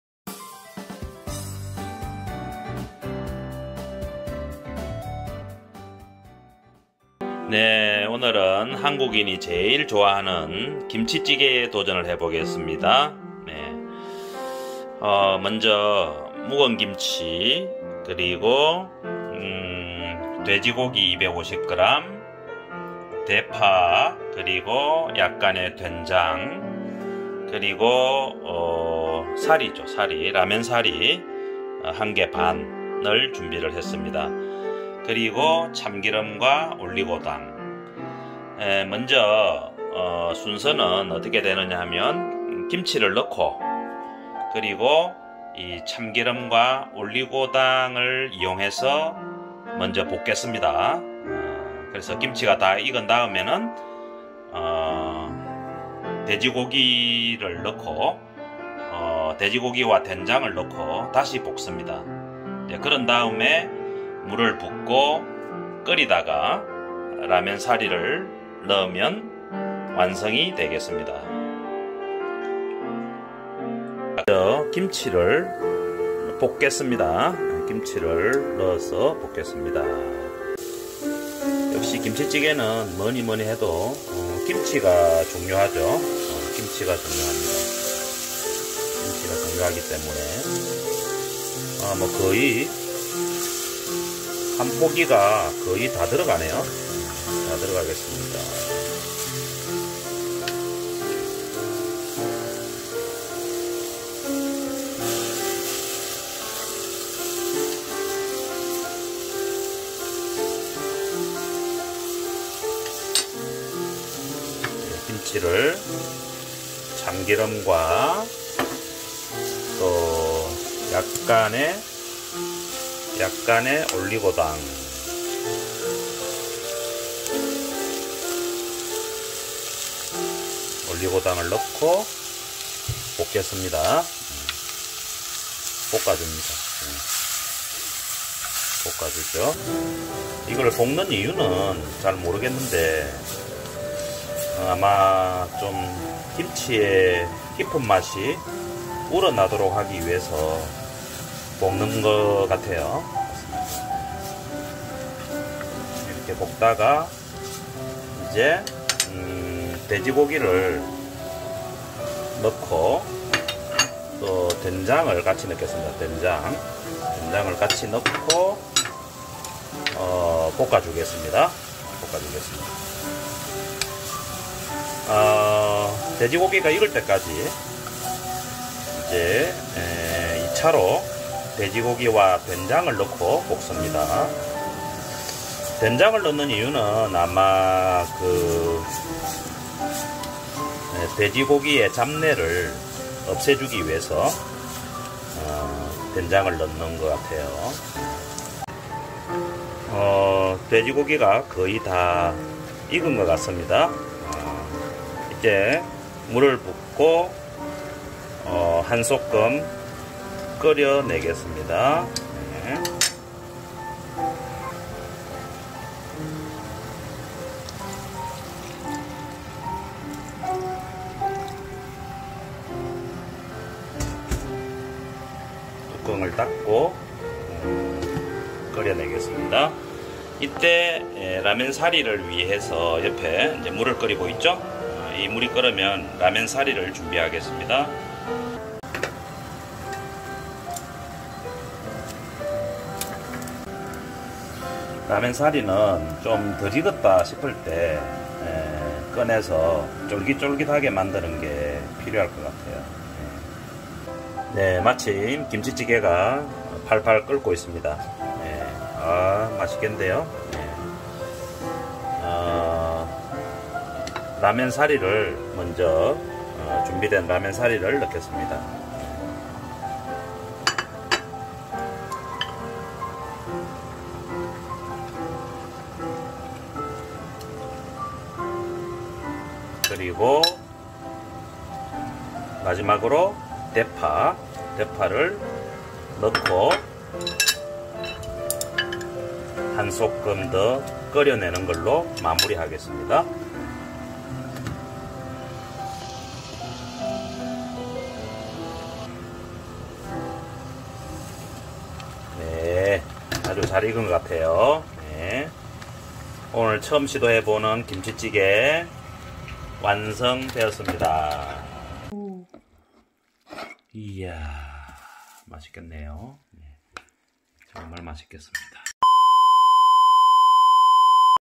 남자의 요리 4 김치찌개
한국인이 제일 좋아하는 대표 요리인 김치찌개에 도전했다.
묵은 김치의 깊은 맛, 돼지 고기의 담백함이 어우러진 한국 대표 음식이다.
여기에 라면사리를 추가하면 한끼 식사로서  이보다 좋기도 어렵다.
라면사리는 살짝 익힌 후 바로 꺼내는 것이 중요하다.
돼지고기는 주로 목살을 많이 쓴다.